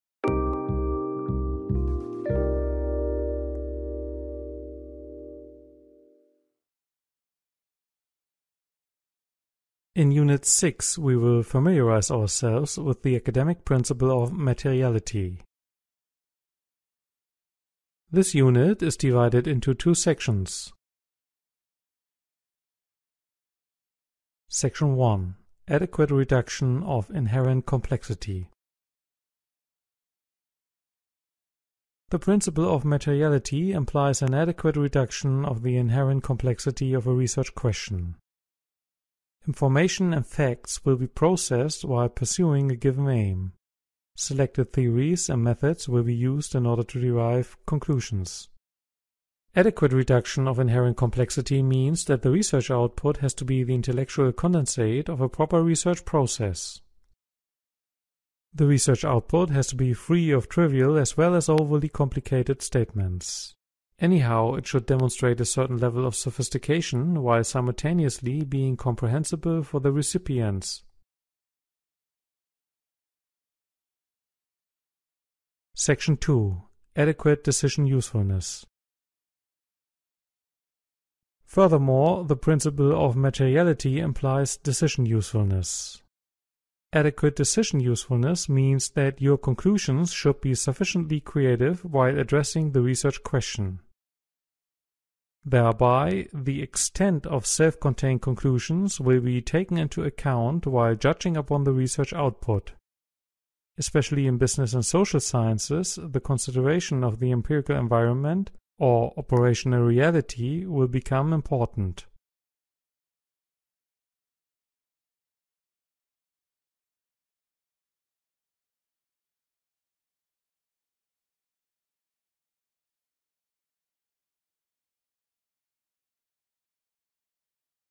Academic research and writing – Chapter 2 Academic principles – Unit 6 Materiality
Academic research and writing
Chapter 2 Academic principles
Unit 6 Materiality

Abstract:

In chapter 2 five major academic principles are introduced. They represent sets of unwritten rules prescribing a certain formal and material behaviour in academic research and writing. Although there is often no legally binding rule set in academic research and writing, certain conventions have been developed and generally accepted by academia. The five major principles presented here are a way to collect dispersed rules and standards and organise them in a systematic way. The five principles are defined as accuracy, completeness, clarity, comparability and materiality. The principle of accuracy implies the observation of applicable rules and norms, the inter-subjective comprehensibility of the research output, the timeliness and currentness of processed information as well as the true and fair representation of the research project. The principle of completeness demands qualitative completeness and quantitative completeness. The principle of clarity requires the clearness and the proper composition of academic texts. The principle of comparability implies obeying the status quo of the discipline and explaining deviations. The principle of materiality prescribes an adequate reduction of inherent complexity and demands adequate decision usefulness for the recipients of a research project.

Academic principles, accuracy, completeness, clarity, clearness, comparability, falsifiability, inter-subjective comprehensibility, materiality, perceptibility, proper composition, reliability, traceability, validity